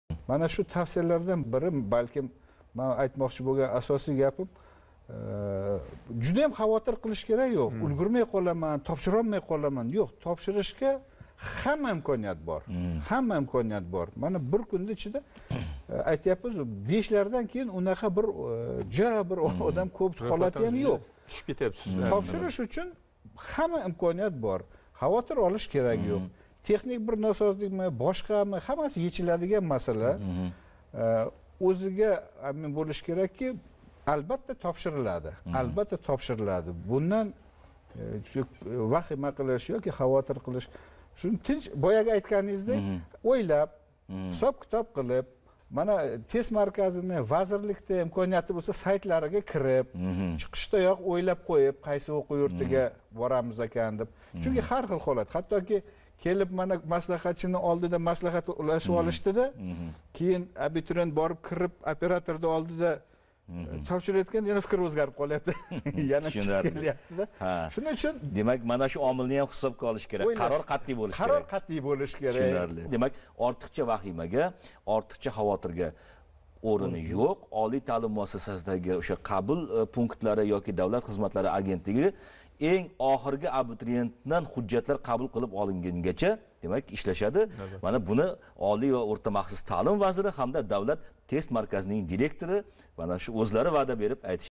OTMlarda ro'yxatdan o'tish bo'yicha vazir tavsiyasi
Абитуриентларга олий таълим муассасасига ҳужжат топшириш (рўйхатдан ўтиш) бўйича вазир тавсияси.